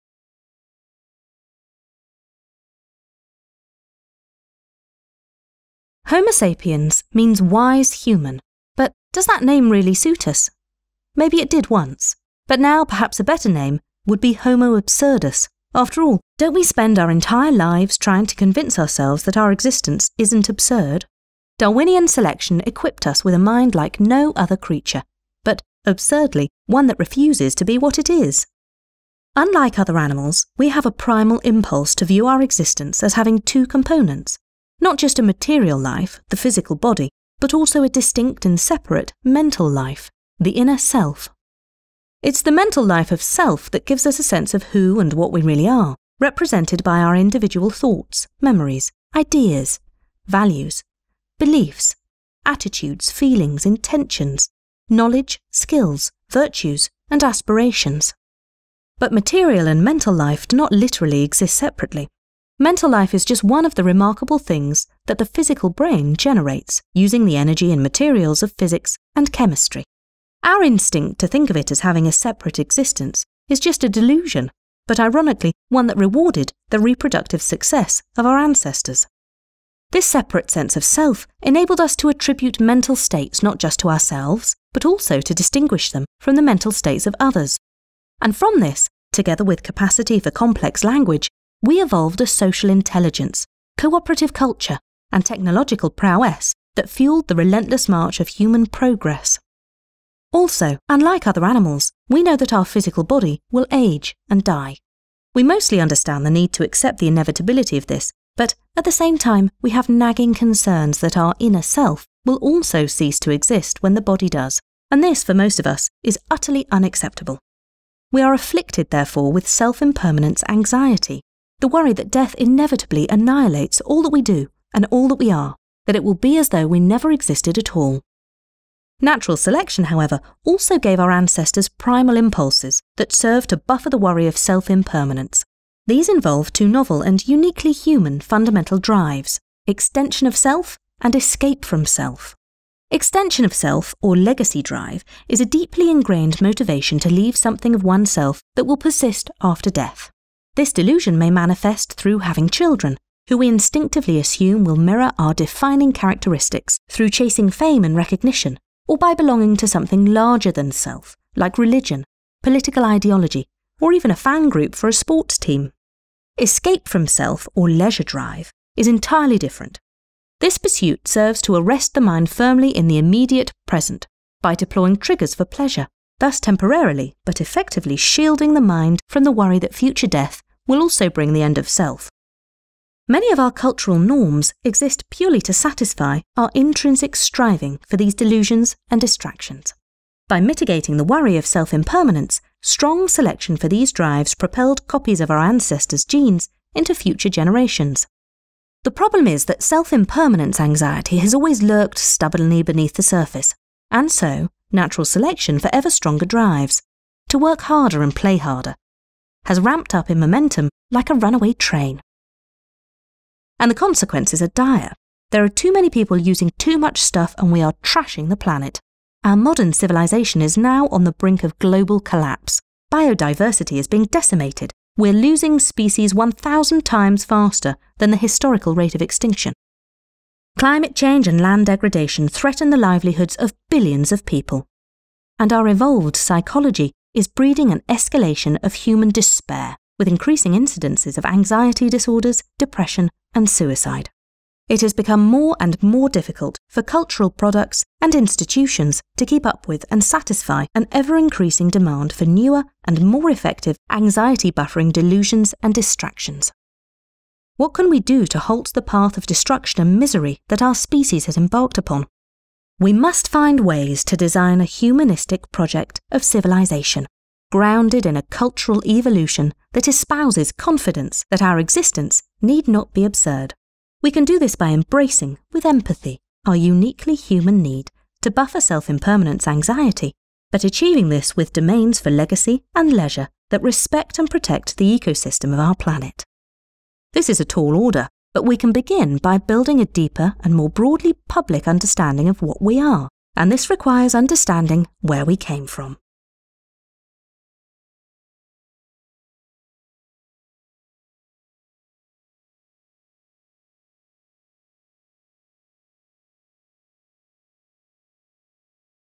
Meet Homo absurdus — the only creature that refuses to be what it is
This video interprets the evolution roots of what draws us to delusions of legacy and distractions of leisure, and how a deeper and more broadly public understanding of — and empathy for — these intrinsic drives will help us in responding effectively to the challenges of our collapsing civilization in the 21st century.  Lonnie Aarssen is a professor of Biology at Queen’s University, Kingston, ON, Canada. His work explores how evolutionary thinking can affect our understanding of our lives, our species and our ability to share the planet with other species.
Additional Information
Aarssen, L. (2010) Darwinism and meaning.  Biological Theory 5: 296 – 311.
Aarssen L. (2015) What Are We? Exploring the Evolutionary Roots of Our Future. Kingston: Queen’s University.
Aarssen L. (2017) The sapiens advantage. Ideas in Ecology and Evolution. 10: 6-11.
Aarssen L. (2018) Meet Homo absurdus —the only creature that refuses to be what it is. Ideas in Ecology and Evolution. 11: 90–95.
Aarssen L. (2019) Dealing with the absurdity of human existence in the face of converging catastrophes.  The Conversation, May 1, 2019.